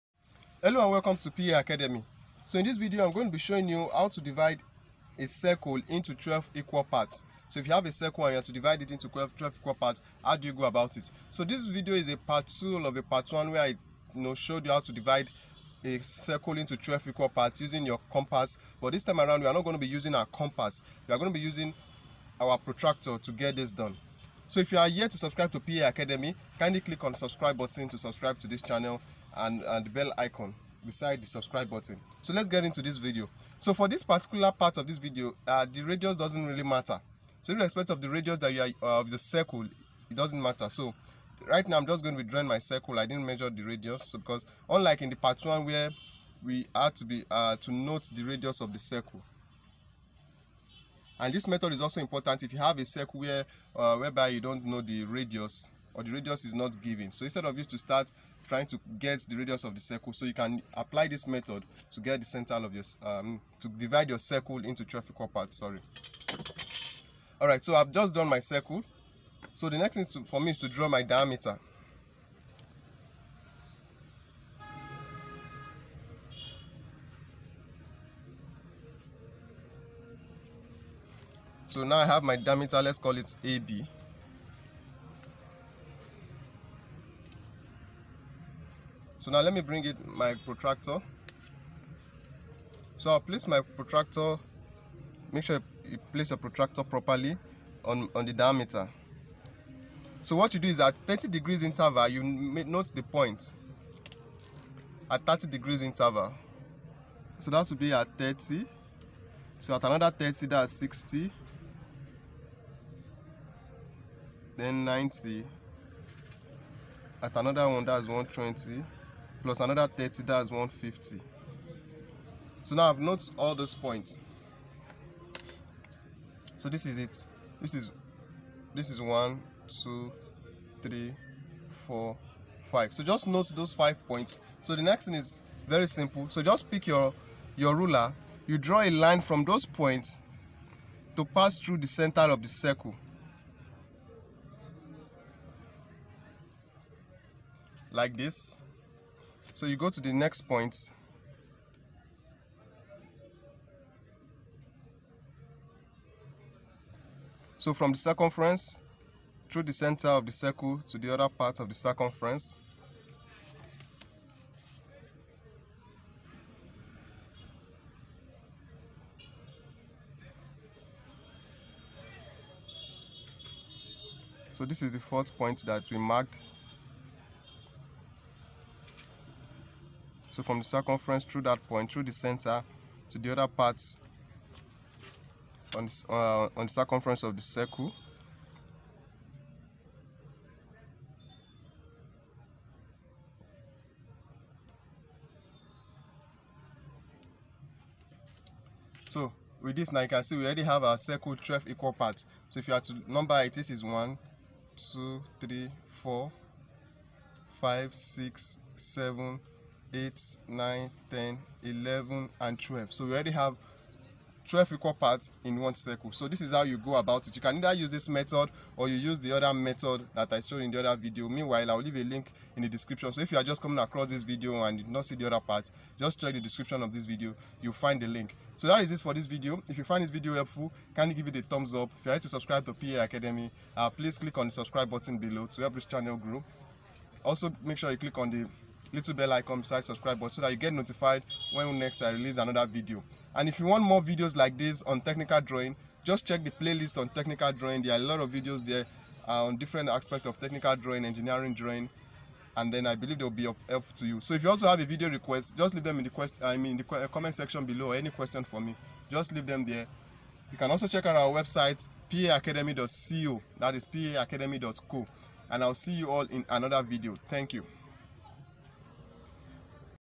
How to Divide a Circle into 12 Equal Parts Using a PROTRACTOR | Divide a Circle WITHOUT a Compass
In this video, I will show you How to Divide a Circle into 12 Equal Parts Using a Protractor. Very simple and easy to do!

Thanks
PA Academy